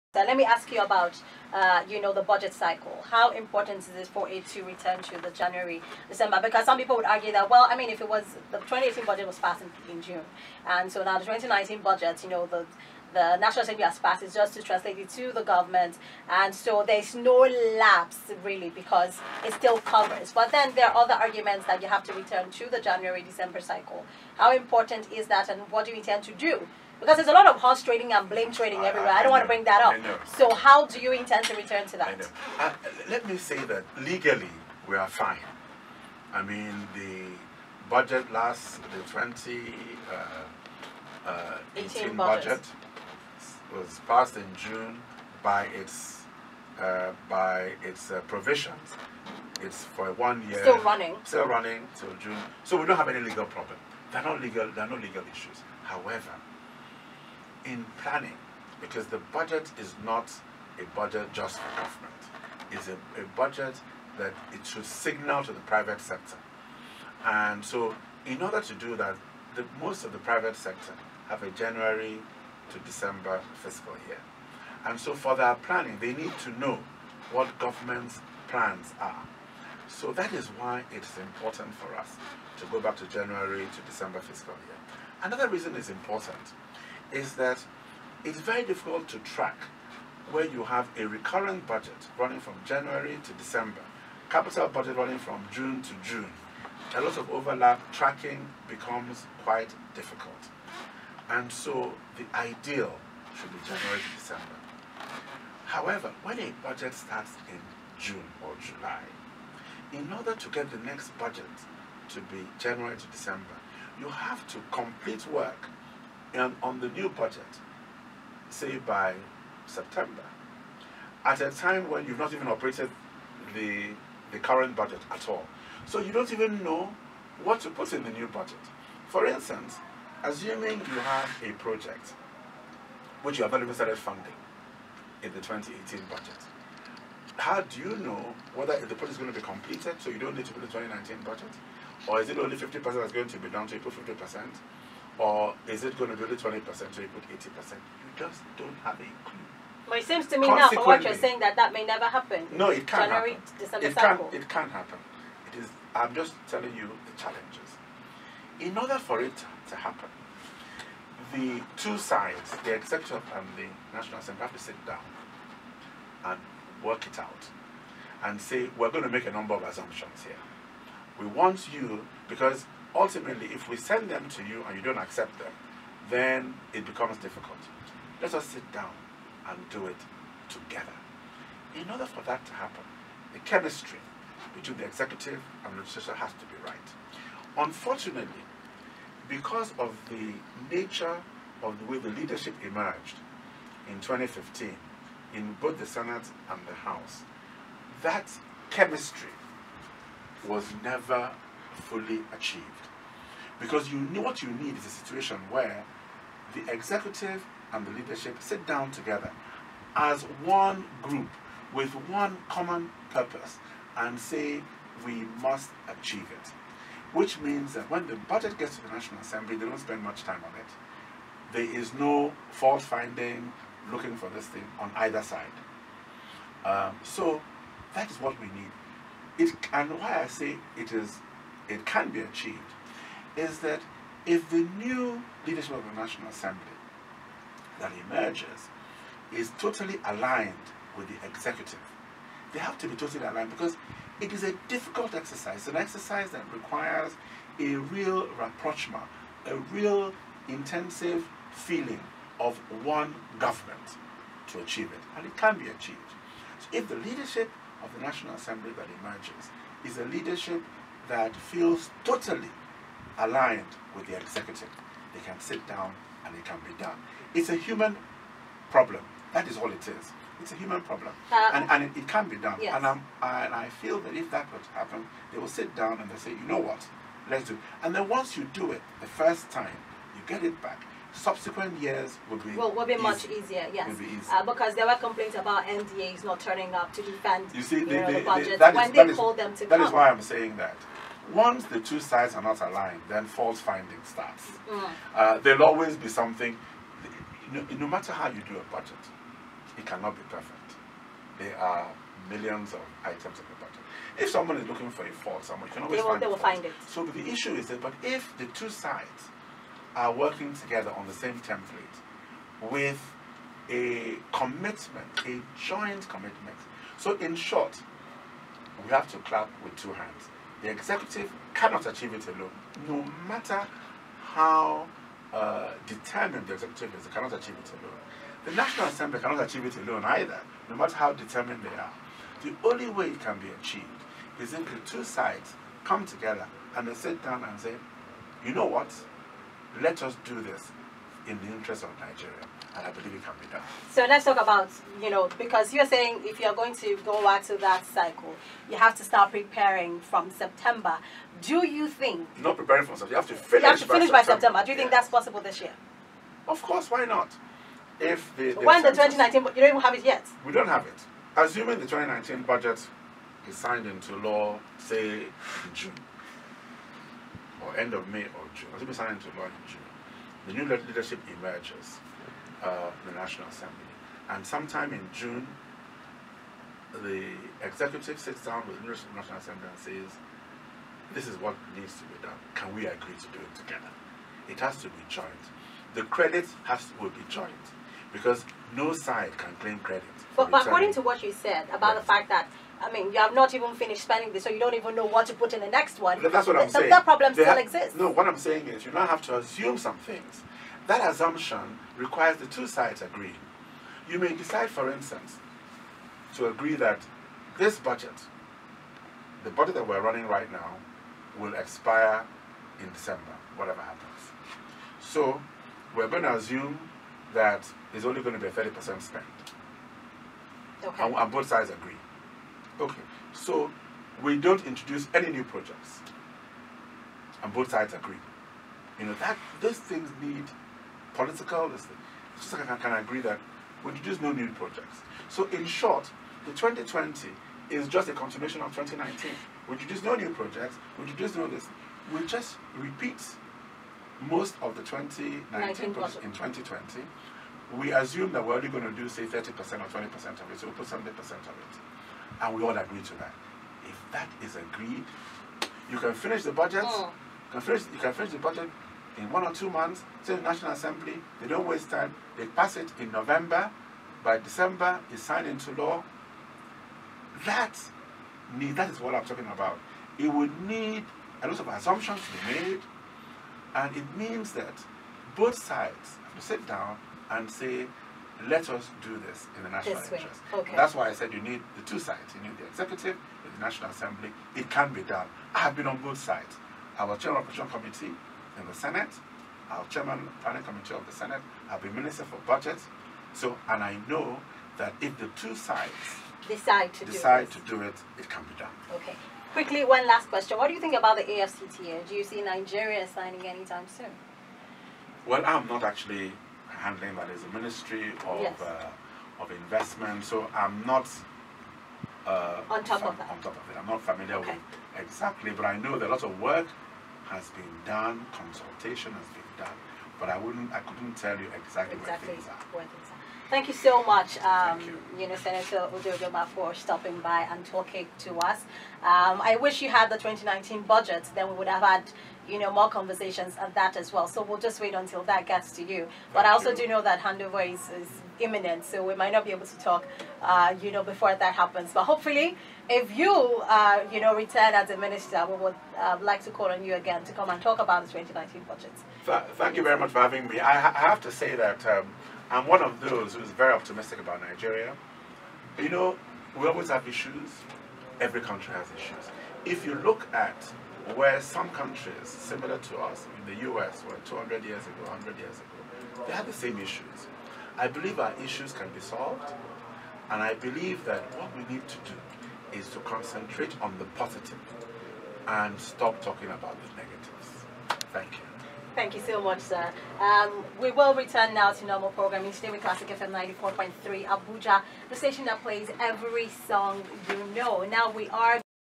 Budget Minister Udo Udoma on Returning to JAN-DEC Budget Cycle
The Honourable Minister, Budget & National Planning, Senator Udoma Udo Udoma was in the Situation Lounge with Faith Dafe Joseph.

He talks about the possibility of a January to December budget cycle.